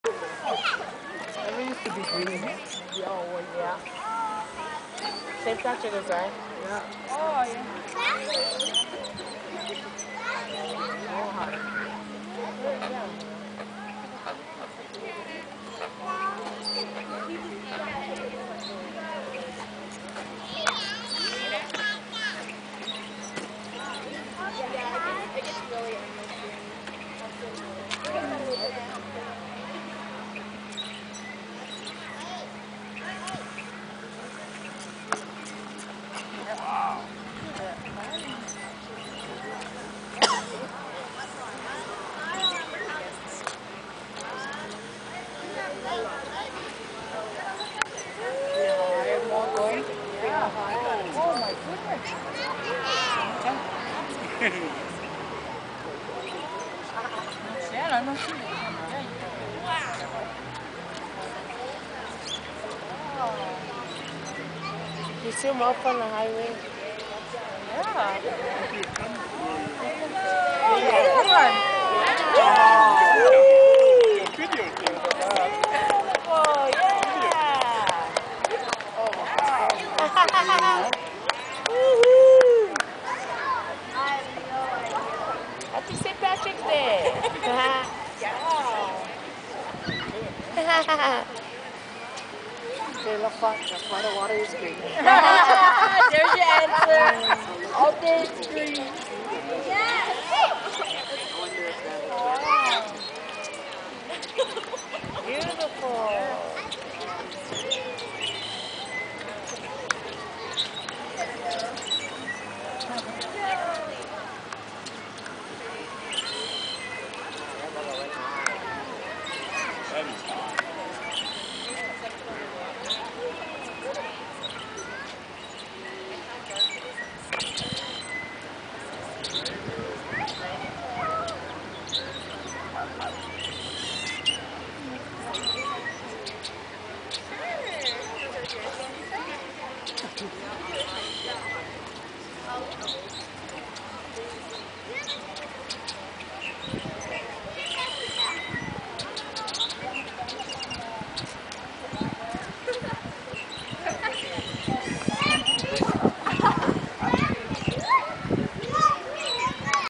St. Patrick's Day Fountain
St. Patrick's Day at the Fountain in Fountain Hills Arizona.